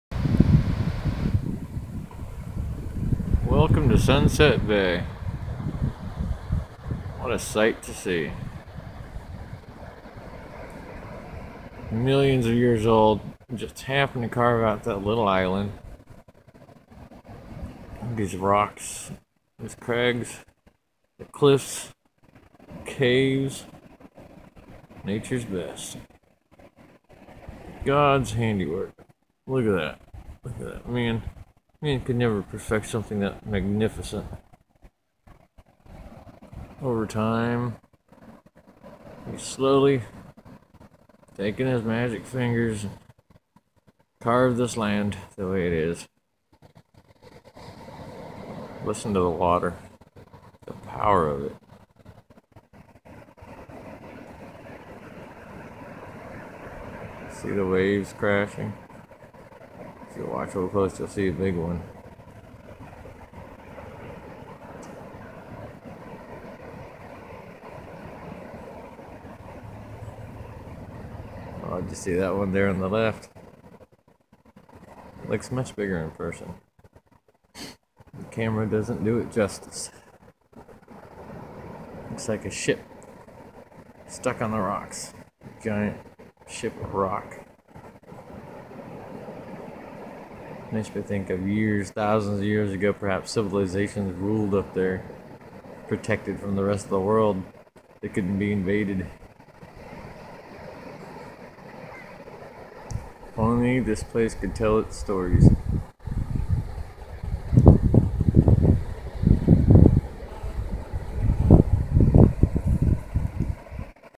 Sunset Bay, Oregon
A narrated tour of Sunset Bay, Oregon